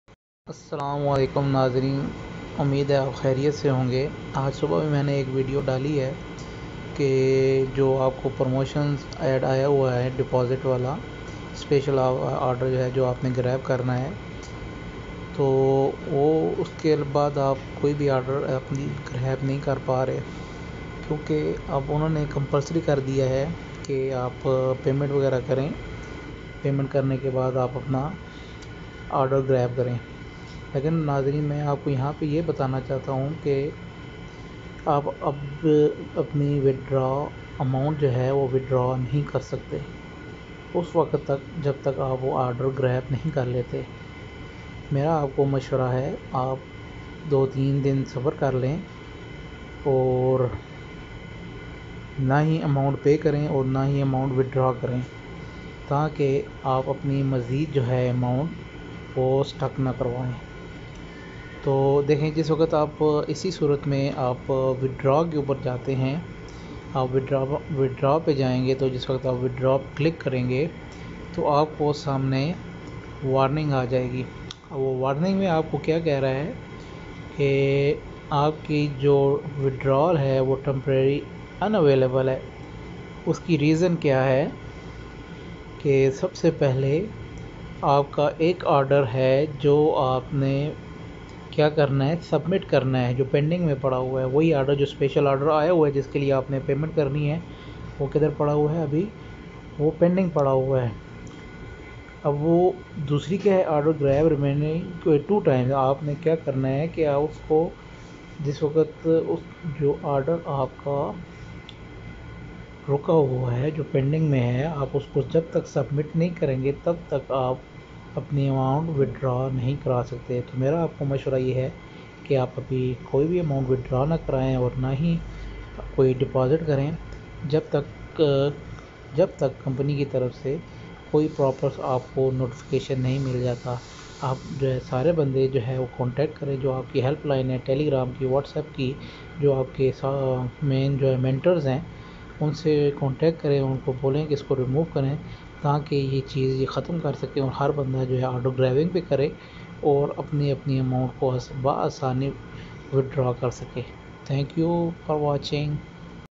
ordergo Warning! Restriction on Withdrawal of deposit amount
Due to update Odergo make new changes for theirs customers. Regarding special order grab and restiction on Withdraw